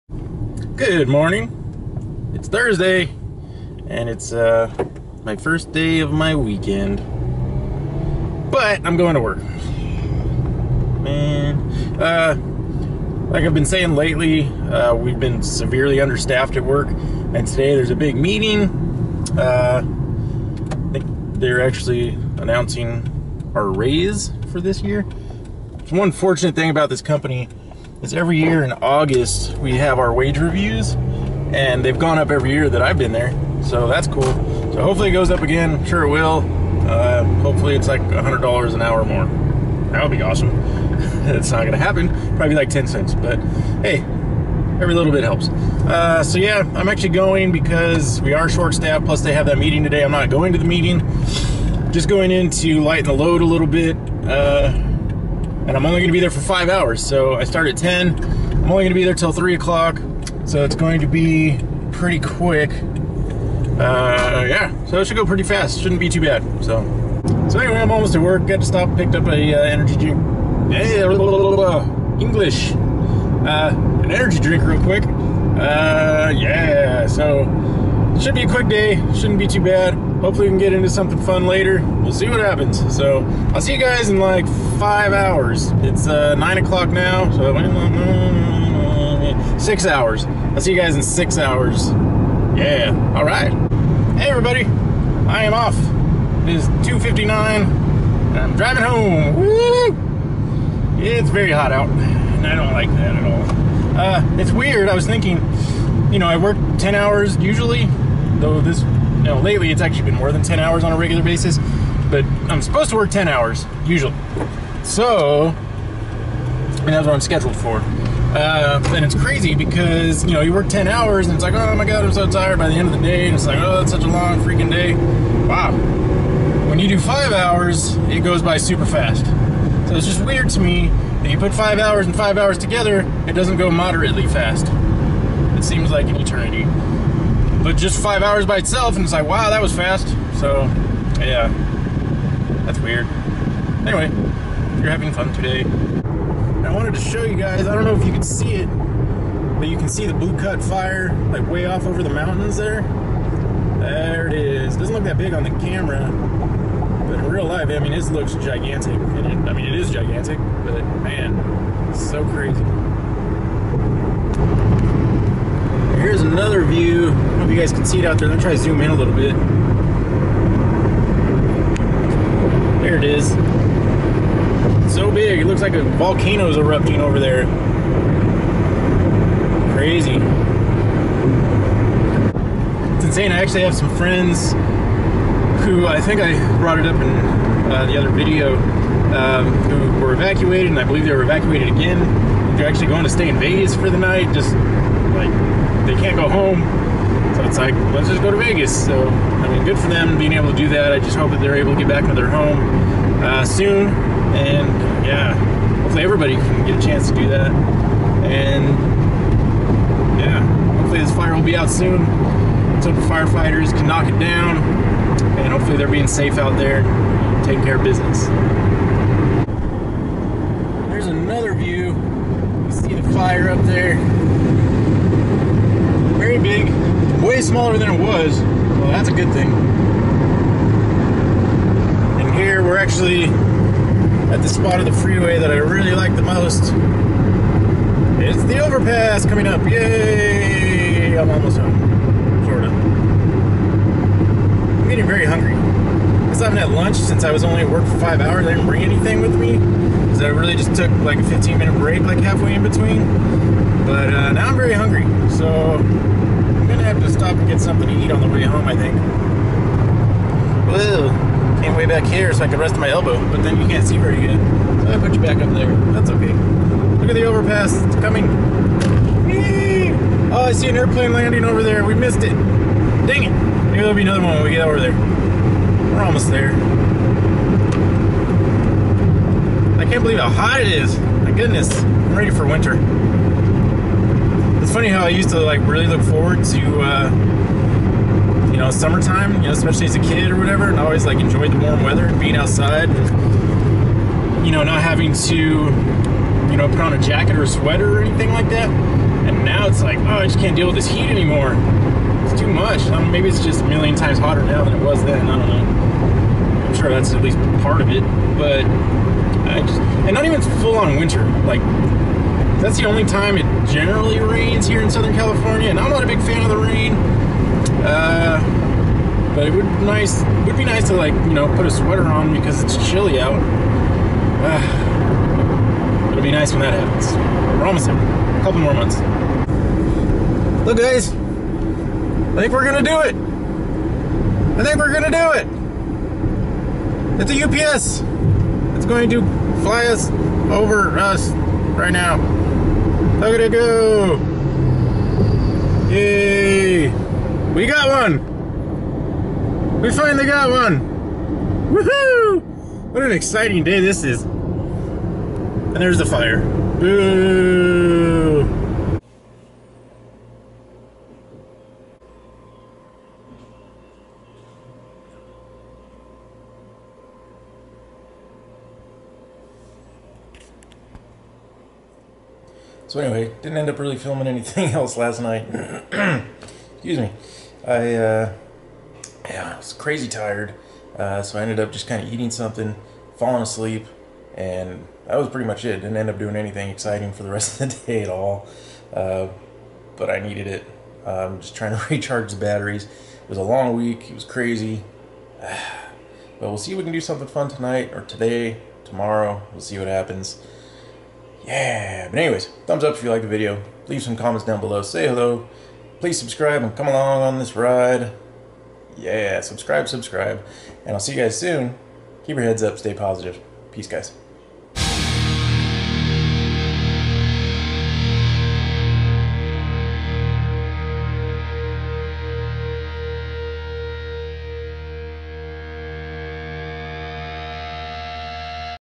Ep. 74: Flying a UPS plane over a fire.
Picked up 5 hours of OT at work. Then I finally caught I plans landing at the airport. Also, a visual update on the Blue Cut Fire. Plus, an adorable cat face.